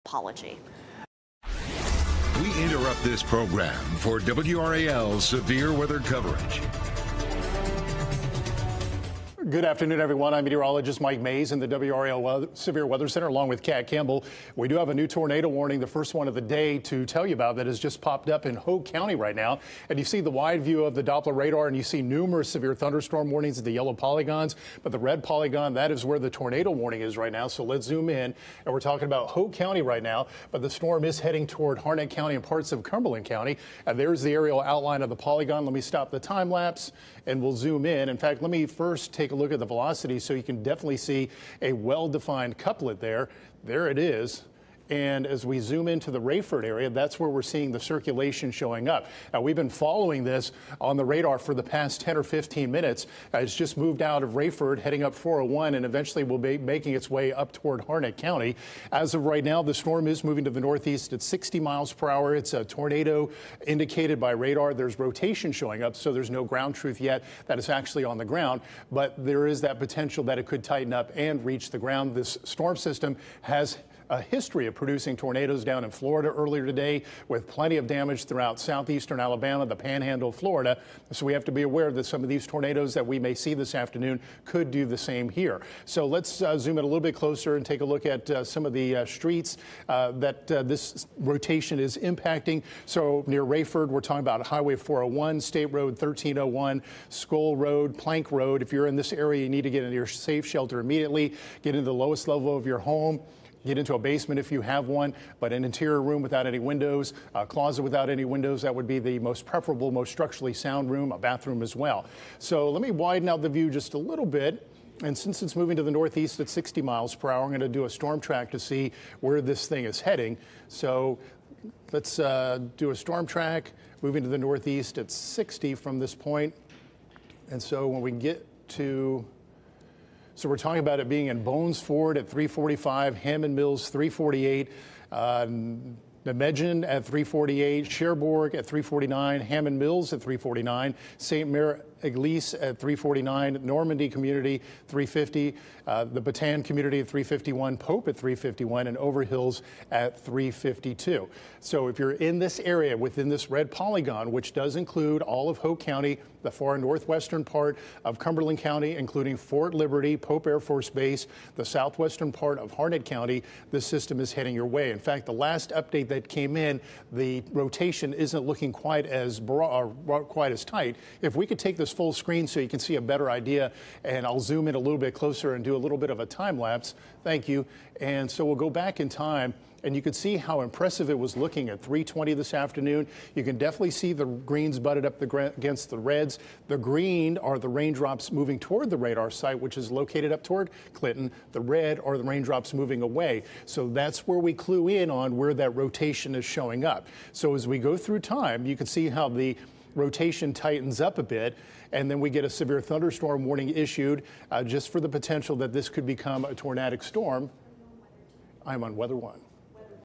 Tornado warning issued for parts of Cumberland, Harnett, Hoke, Lee and Moore counties
Tuesday is a WRAL Weather Alert Day due to the threat of heavy rainfall, damaging winds, flooding and isolated tornadoes. The Triangle is under a Level 2 risk for severe weather, and multiple counties southeast of Raleigh, including Cumberland and Harnett, are under a Level 3 risk for severe weather due to a line of storms moving in from the west.